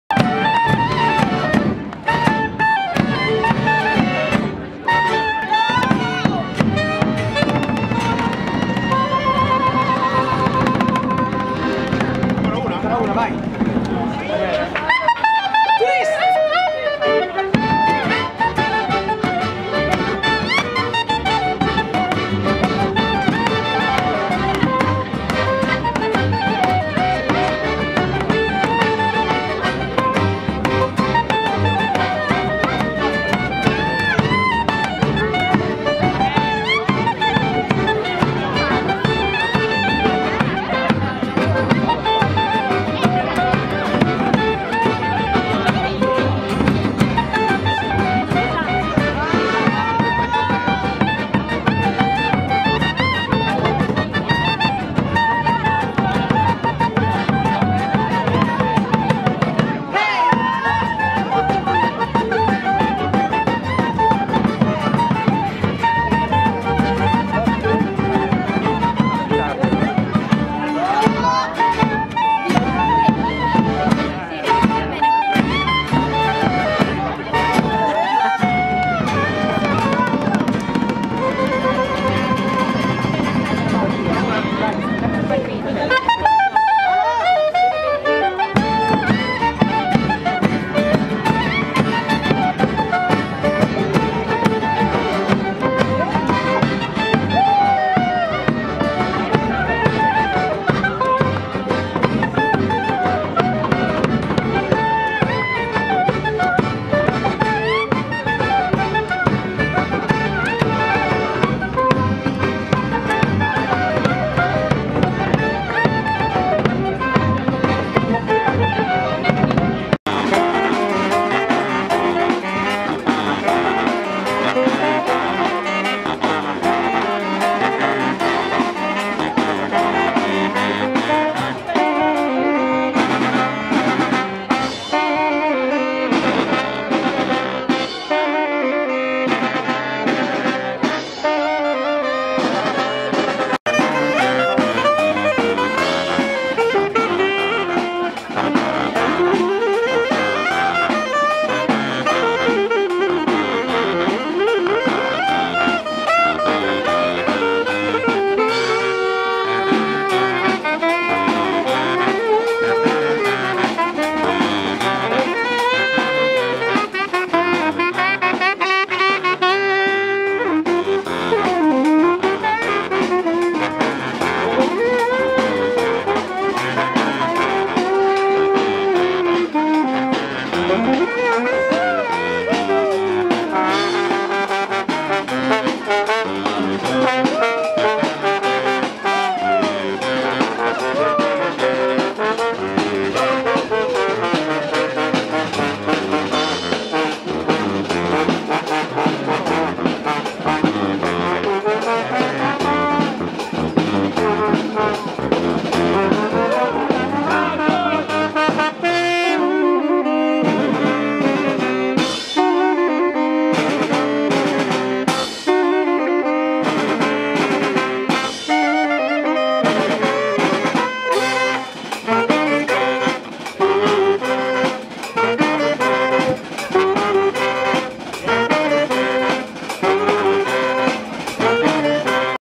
Festa della Birra a Expo 2015
Musica nella corte di Cascina Triulza per la Festa della Birra il 19 settembre 2015